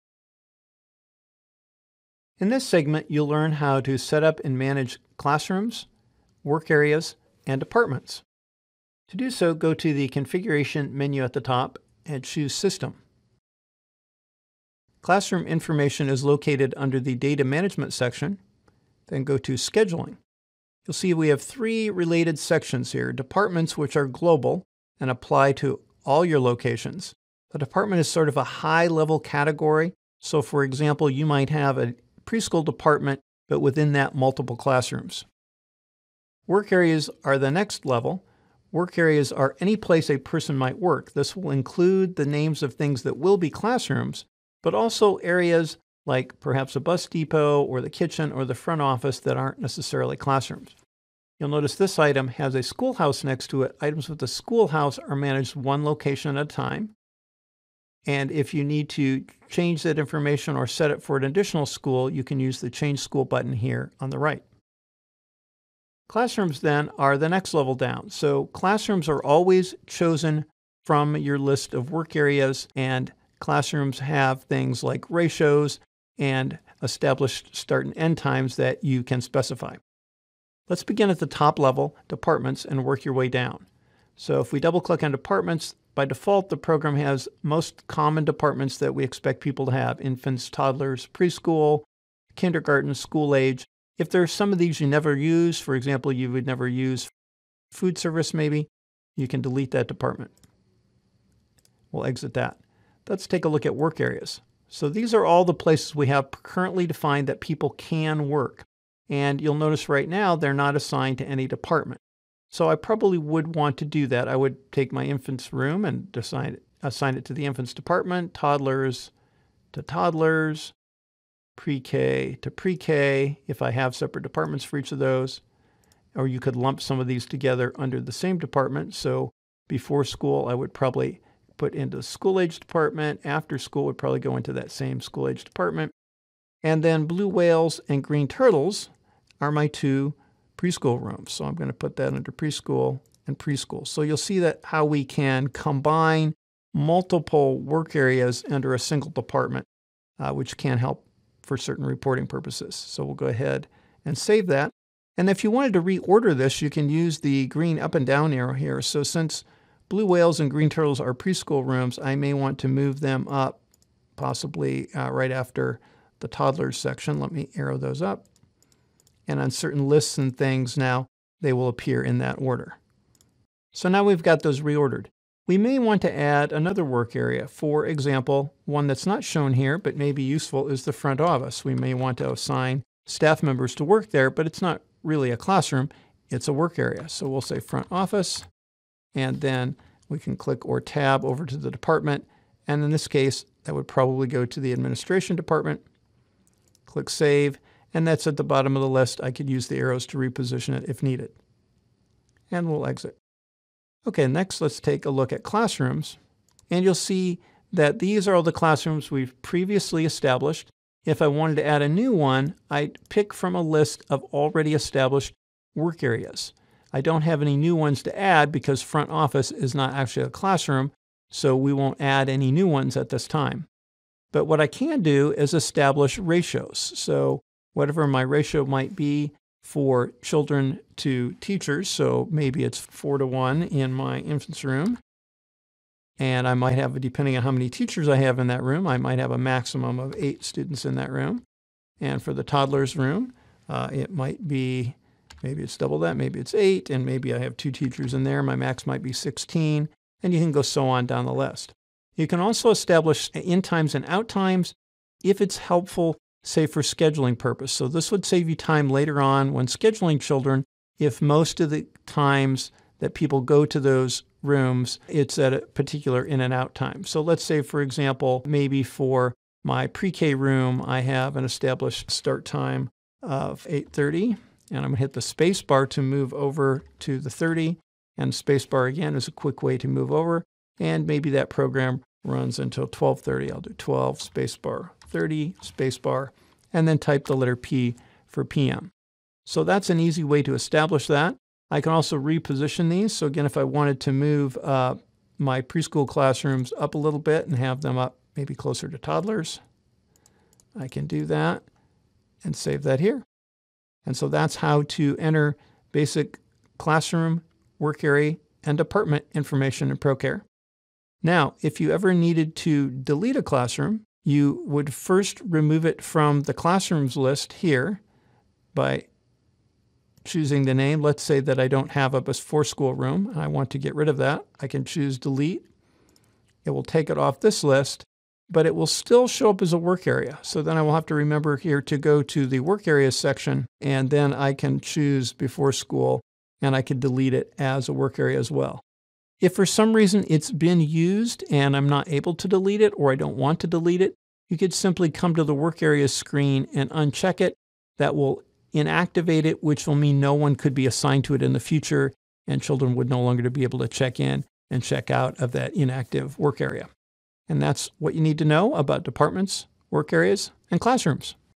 Procare Desktop: Set Up Classrooms
How to set up classrooms in Procare.